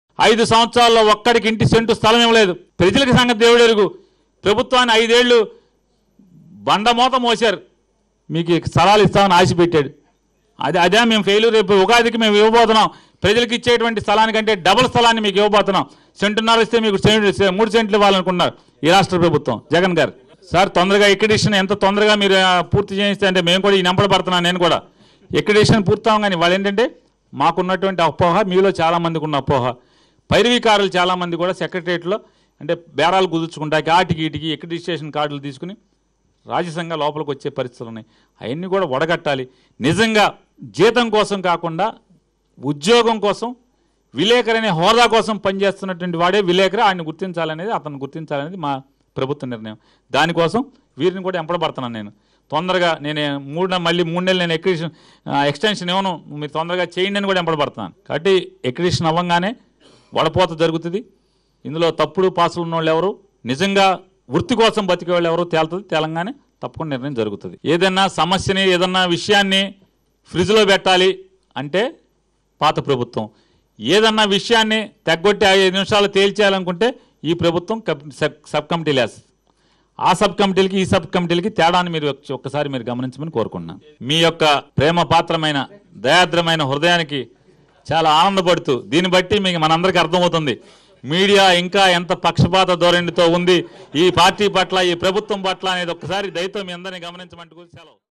ప్రజలతో పాటు జర్నలిస్టులకు ఇళ్ల స్థలాలు|psknews
అమరావతి సచివాలయం లో మంత్రి పేర్ని నాని మీడియా సమావేశం...వివరాలు

అంధ్రప్రదేశ్ లో జర్నలిస్టు లకు జగన్మోహన్ రెడ్డి ప్రభుత్వం  ఉగాది కానుక

ఉగాదినాడు ప్రజలతో పాటు జర్నలిస్టులు కూడా ఇళ్లు స్థలాలు

ప్రజలకిచ్చే దానికన్నా రెట్టింపు స్థలం... ఒక్కొక్క జర్నలిస్టు కు మూడు సెంట్లు ఇళ్ళ స్థలం..ఇవ్వనున్న ప్రభుత్వం

అక్రిడెషన్లు పొడిగింపు లేకుండా సత్వరమే నూతన అక్రిడేషన్ లు మంజూరు చేసి దాని ఆధారంగా ఇళ్లు స్థలాలు మంజూరు